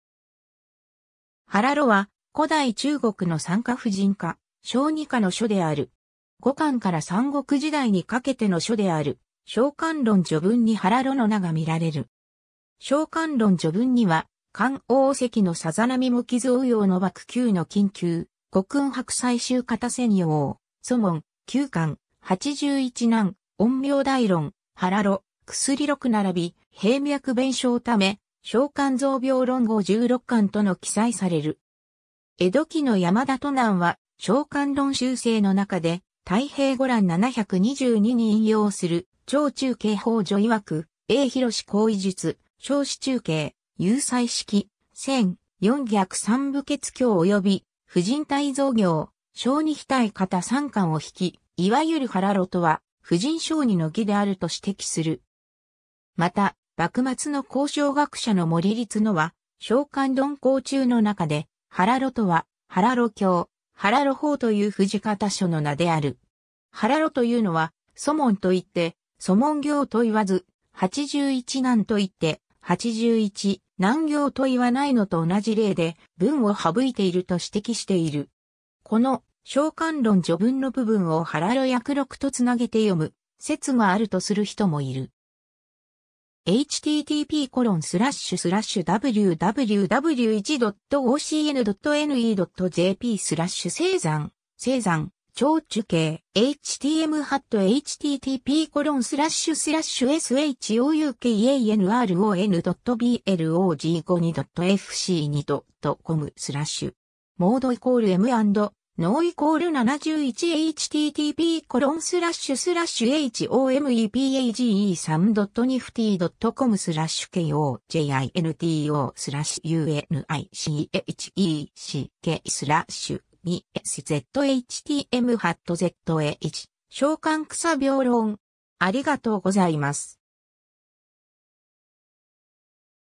胎臚
胎臚（たいろ）は、古代中国の産科婦人科・小児科の書である。
 後漢から三国時代にかけての書である『傷寒論』序文に胎臚の名がみられる。
『傷寒論』序文には、「感往昔之淪喪　傷横夭之莫救　乃勤求古訓　博采衆方　撰用『素問』　『九巻』　『八十一難』　『陰陽大論』　『胎臚』　『薬録』　并『平脈辨証』　為『傷寒雑病論』　合十六巻」との記載される。
 江戸期の山田図南は『傷寒論集成』の中で『太平御覧』七百二十二に引用する「張仲景方序曰、衛汎好醫術、少師仲景、有才識、撰『四逆三部厥経』及『婦人胎蔵経』『小児顱顖方』三巻」を引き、「いわゆる胎臚とは婦人小児の義である」と指摘する。
 また、幕末の考証学者の森立之は、『傷寒論攷注』の中で「胎臚とは胎臚経・胎臚方という婦児方書の名である。胎臚というのは『素問』といって『素問経』と云わず、『八十一難』といって『八十一難経』といわないのと同じ例で、文を省いている」と指摘している。
 この『傷寒論』序文の部分を胎臚薬録とつなげて読む説があるとする人もいる。